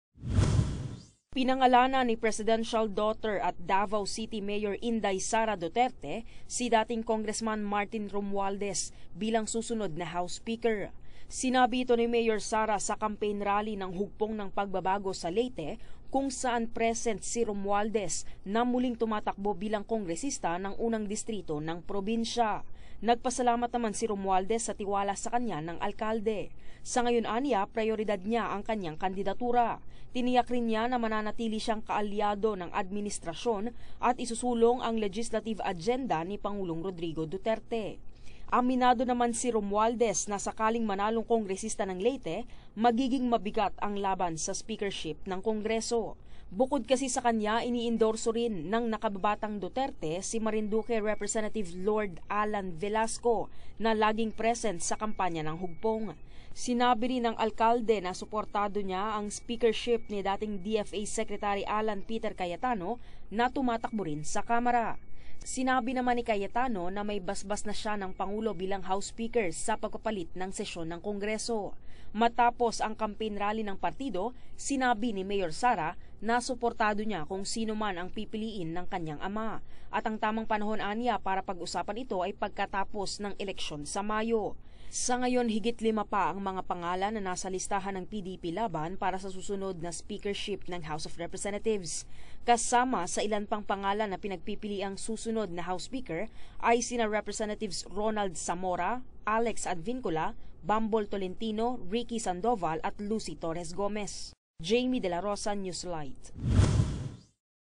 SARA DUTERTE, INENDORSO SI MARTIN ROMUALDEZ BILANG SUSUNOD NA HOUSE SPEAKER | March 20, 2019
PANOORIN:

Inendorso  ni Presidential Daughter at Davao Mayor Sara Duterte si Martin Romualdez bilang susunod na House Speaker. Si Romualdez ay kumakandidato bilang kongresista ng unang distrito ng lalawigan ng Leyte.